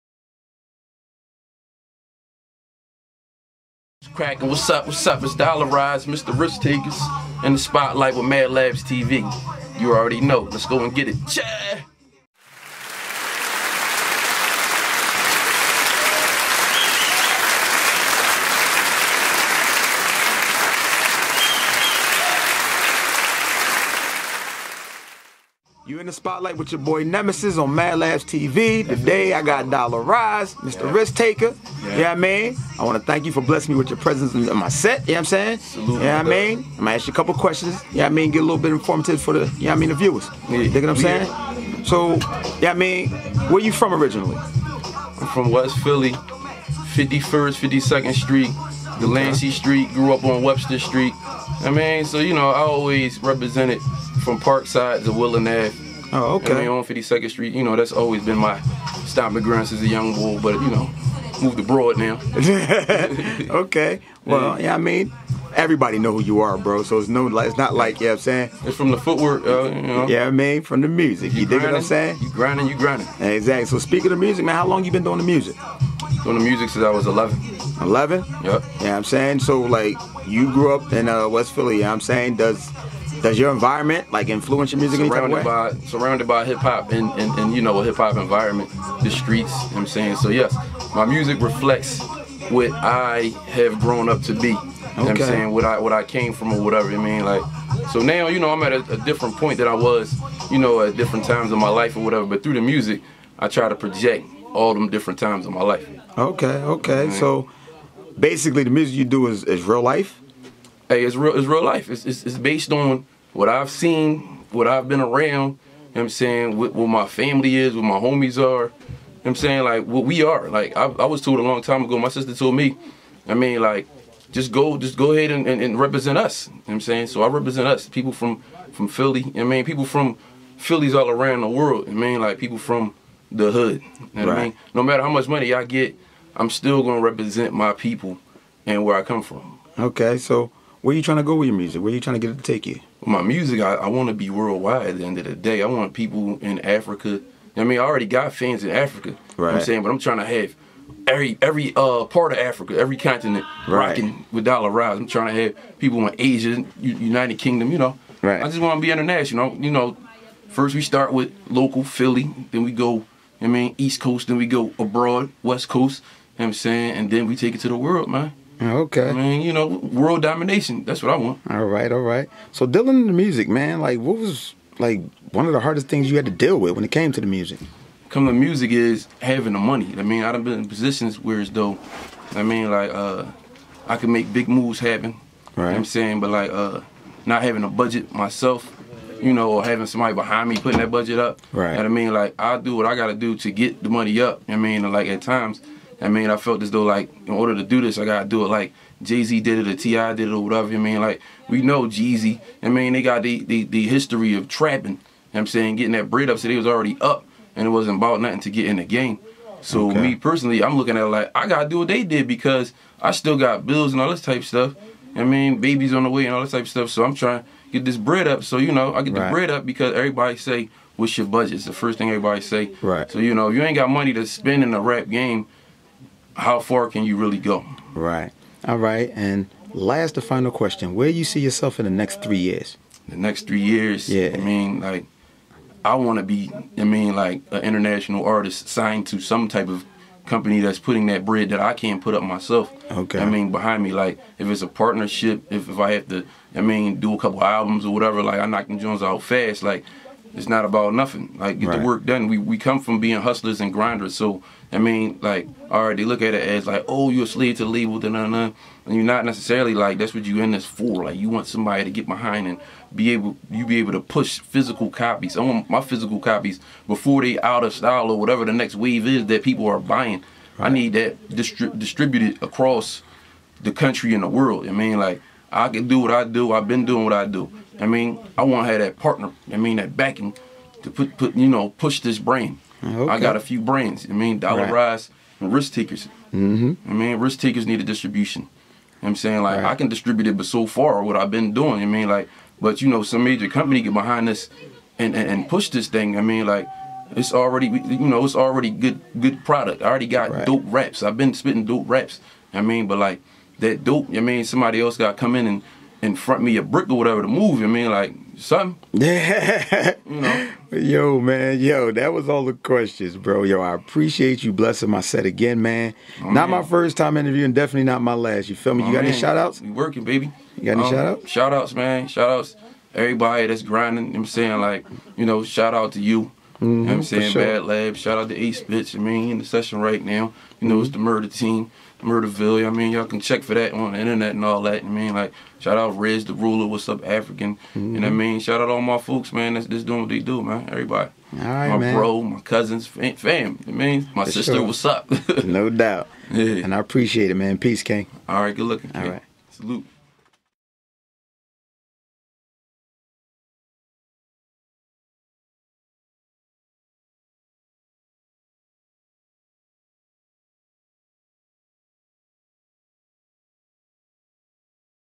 MADD LABS TV IN THE SPOTLIGHT WITH DOLLA RISE HOSTED BY NEMISIS
HERE'S ANOTHER EPISODE OF MADD LABS TV IN THE SPOTLIGHT WITH SPECIAL GUEST DOLLA RISE CHECK IT OUT AS HE TALKS ABOUT HIS UP AND COMING PROJECTS AND OTHER VENTURES TUNE IN! SUPPORT THE ARTIST